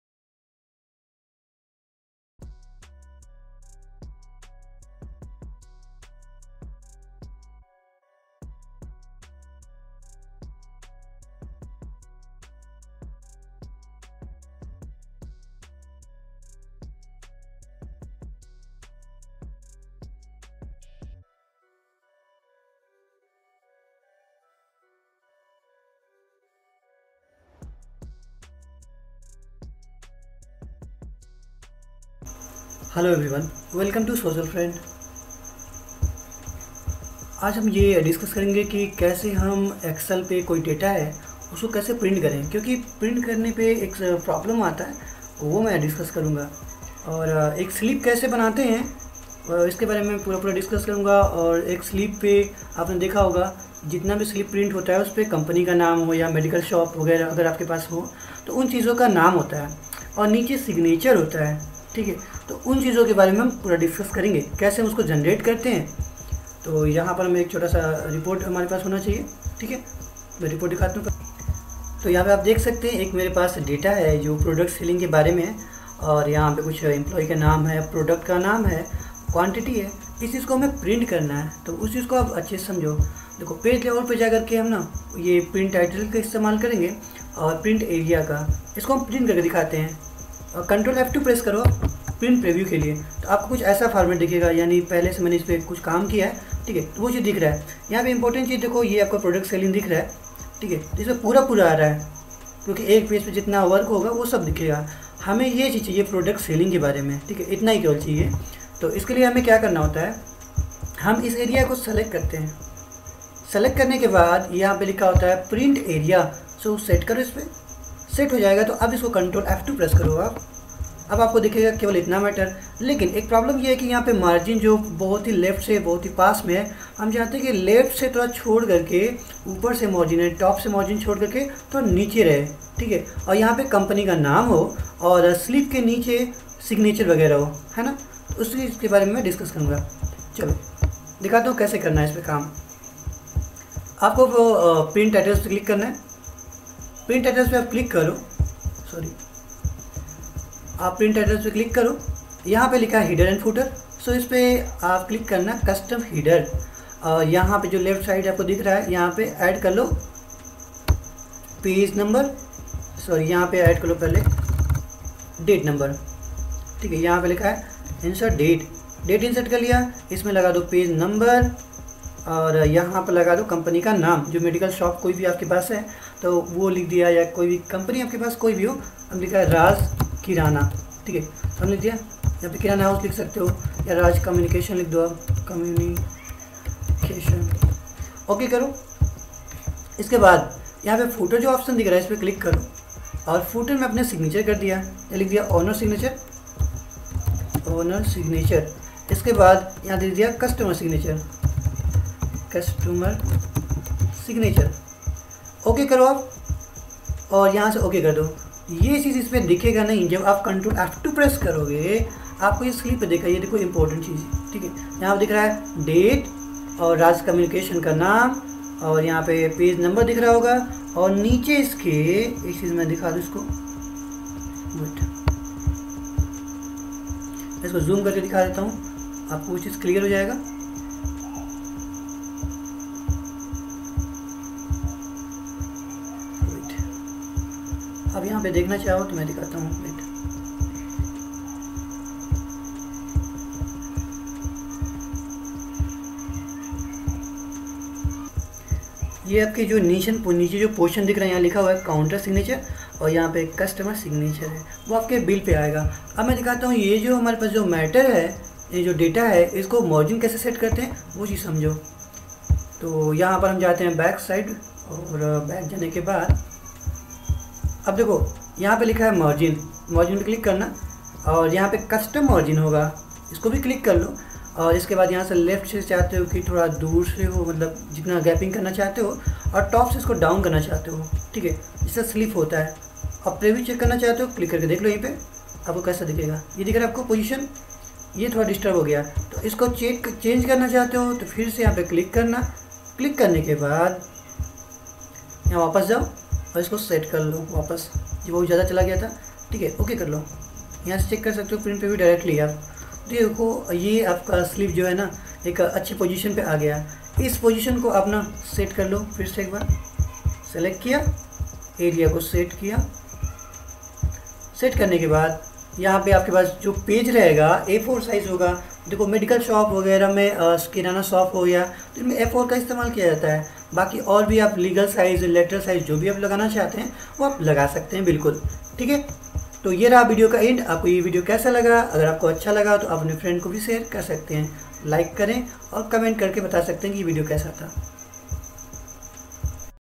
Print Area & Print Titles, Set Margine,Print Invoice in Ms Excel in Hindi
Social FRND